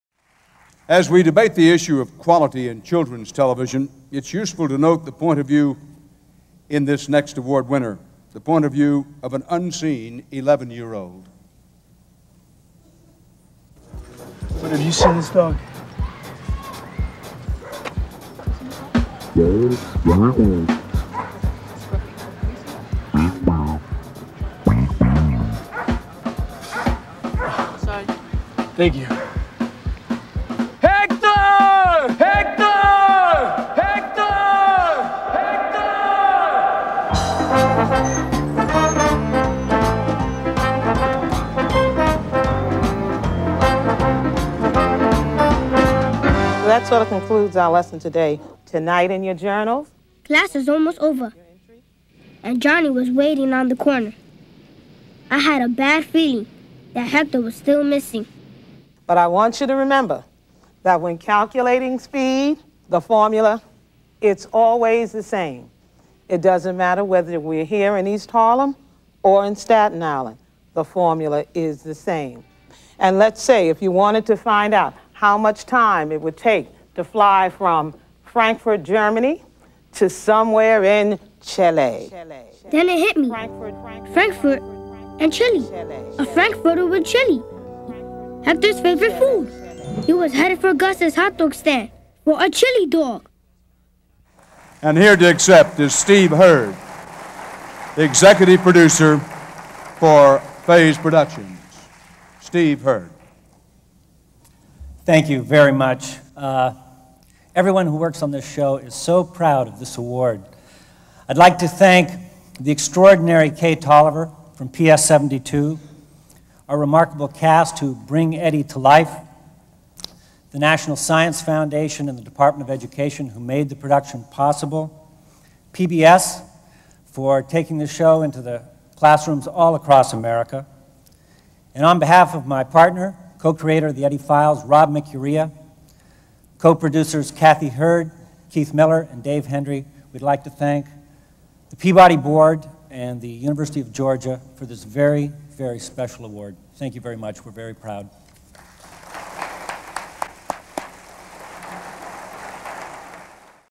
Steven R. Heard - The Eddie Files - 1997 Peabody Award Acceptance Speech
Winner 1997 | FASE Productions for PBS

In the past year, policy makers and programmers have turned increasing attention to children’s television. As the debate rages about mandates for achieving a higher quantity and quality of programming for American children, The Eddie Files offers a model for producers to emulate. Critical to the success of The Eddie Files is its point of view. It has none. To explain, the program unfolds through the subjective eye view of an 11-year-old, who is heard, but never seen on screen. Children thus have their imaginations stimulated and engaged. As his problem-solving adventures unfold, children can project onto the unseen Eddie their own ethnicity, social class and family situations. In this way, they are brought into the learning experience. And what a learning experience it is! Beginning with the extraordinary in-class teaching of mathematics mentor Kay Toliver (the subject of a prior Peabody-winning production), The Eddie Files incorporates innovative and engaging documentary, dramatic vignettes, as well as the broad comedic scenes that children love so much. The Eddie Files is another successful enterprise from the Foundation for Advancements in Science and Education. FASE Productions, led by executive producer and writer Steven R. Heard, senior producer Kathie Heard; producer, director and writer Rob Mikuriya; supervising producer Keith Miller; coordinating producer Dave Hendry, and a talented team of writers, including John T. Davis, Maria Lokken and Jay Dorfmann, is commended for their ongoing record of excellence. This level of quality sustains in the range of support materials, including teacher’s guides and classroom instructional materials. For producing inspiring children’s television, which makes math fun, exciting, and important to children, a Peabody is presented to The Eddie Files.